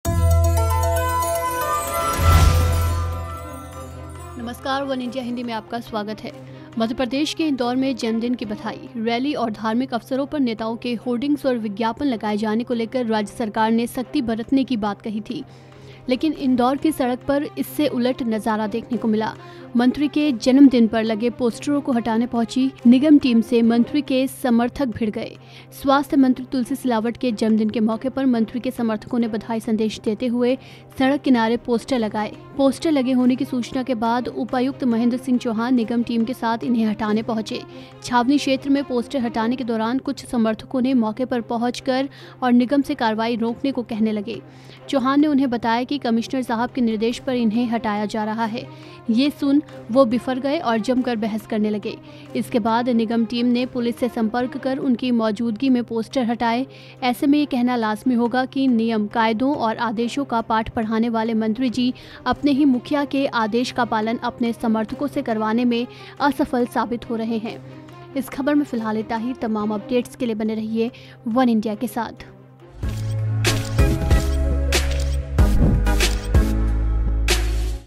Indore में Congress के Tulsi Silawat के Bday poster हटाने गई निगम Team से हाथापाई | वनइंड़िया हिंदी
Indore On Tuesday, minister supporters, who bridged the residency area with banner-flakes on the birthday of Health Minister Tulsi Silavat, disputed and abused municipal officials-employees. While the supporters threw stones at the staff for a long time, some even grabbed the collar of the employees and pushed them.

 स्वास्थ्य मंत्री तुलसी सिलावट के जन्मदिन पर रेसीडेंसी क्षेत्र को बैनर-फ्लैक्स से पाटने वाले मंत्री समर्थकों ने मंगलवार को नगर निगम के अधिकारियों-कर्मचारियों से जमकर विवाद और गाली-गलौज की। समर्थकों ने अमले पर देर तक पत्थर फेंके तो कुछ ने कर्मचारियों की कॉलर पकड़कर धक्का-मुक्की भी की।